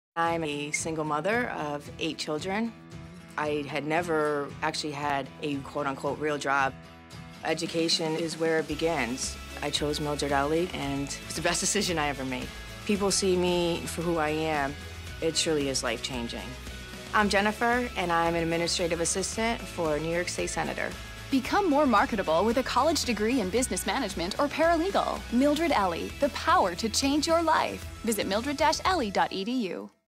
Mildred Elley Business and Paralegal Commercial
The Paralegal Studies certificate program as part of the School of Business & Technology at Albany, NY prepares students with the skills necessary to seek entry-level positions in law offices, title/abstract companies, insurance companies, legal divisions of corporations, banks and legal services companies.

Jobs in Business Management are abundant and the range of businesses is unlimited. Our graduates are well prepared for positions in banking, finance and sales, customer service, and many other for-profit, non-profit, and government jobs. They are trained in the latest computer technologies to enhance their productivity, ensuring them a competitive advantage over other job candidates.